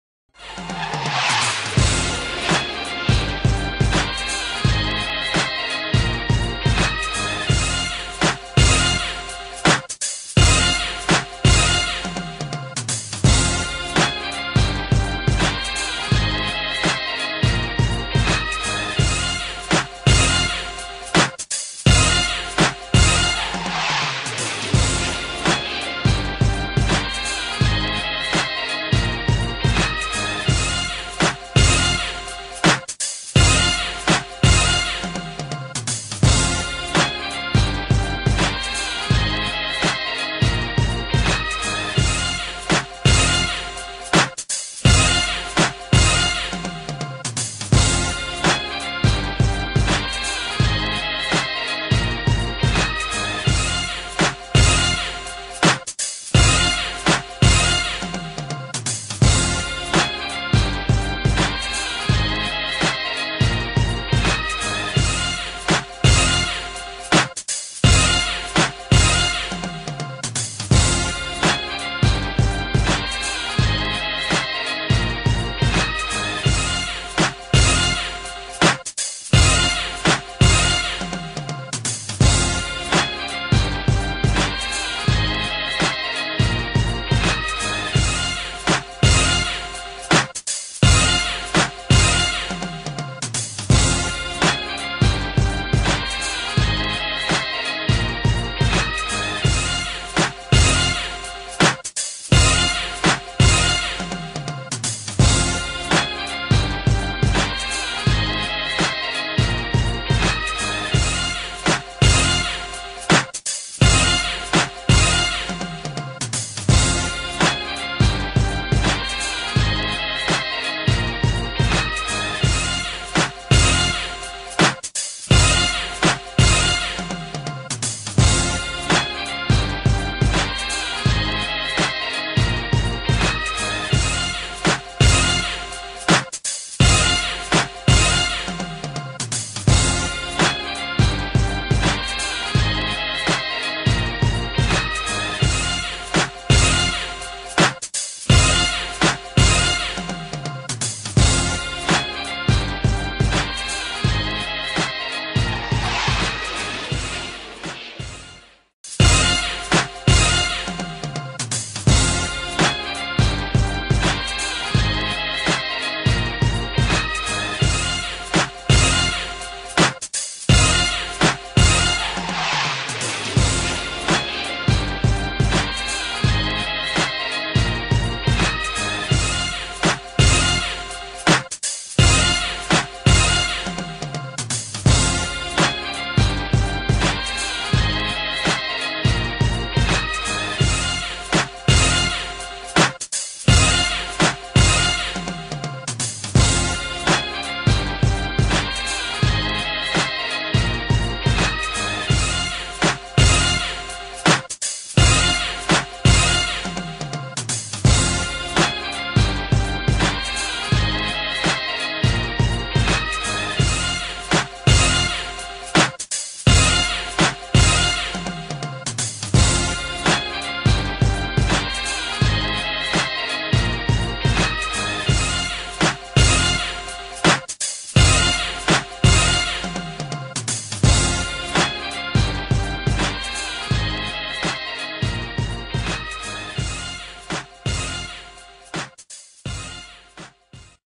Brandon Parker Senior Highlight Film
Brandon Parker Out of Bishop McDevitt Wyncote Senior Highlight Film at the Defensive End Position